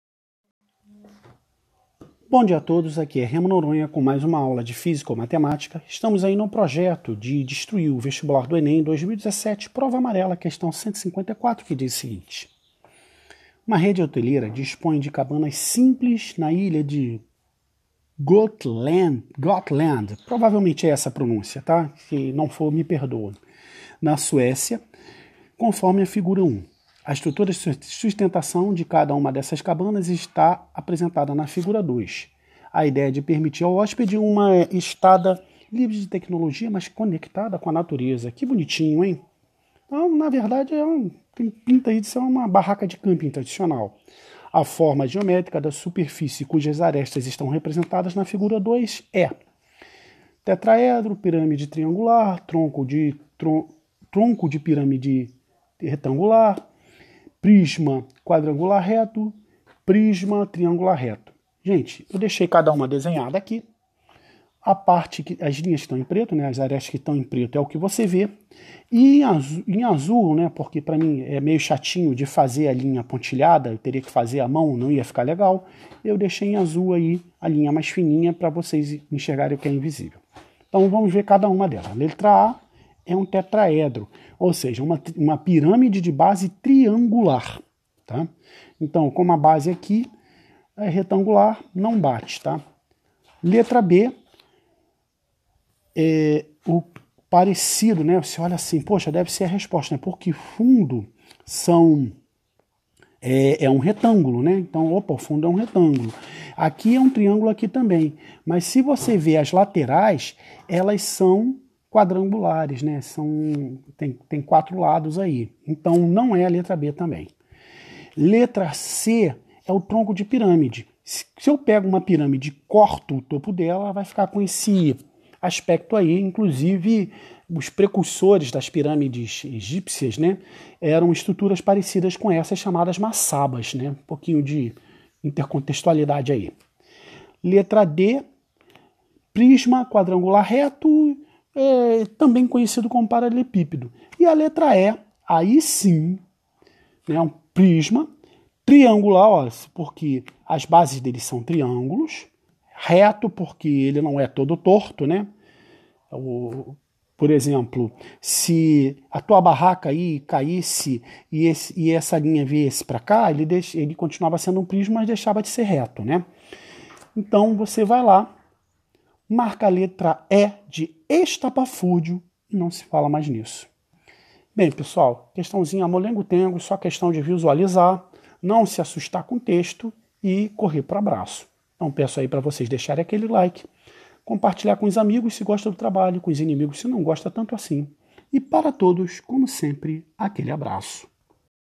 ENEM 2017 prova amarela questão 154, Qual é o formato de uma barraca
Uma rede hoteleira dispõe de cabanas simples na
ilha de Gotland, na Suécia, conforme Figura 1. A estrutura
de sustentação de cada uma dessas cabanas está
representada na Figura 2. A ideia é permitir ao hóspede uma
estada livre de tecnologia, mas conectada com a natureza.
Qual é o formato de uma barraca